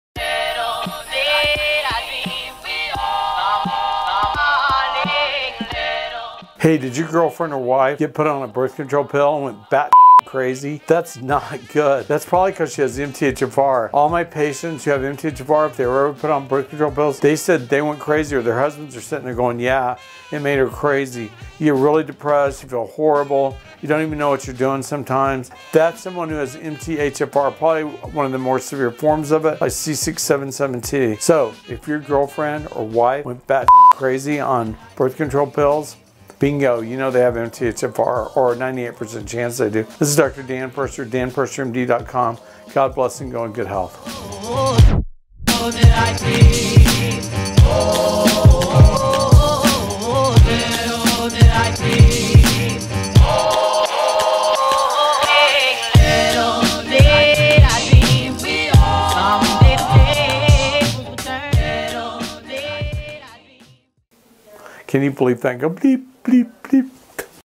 Does Birth Control Make You Feel Crazy?
Dr. Purser talks about the effects birth control has on his patients with MTHFR. If birth control has made you feel weird, consider getting tested for the MTHFR gene to start feeling normal again!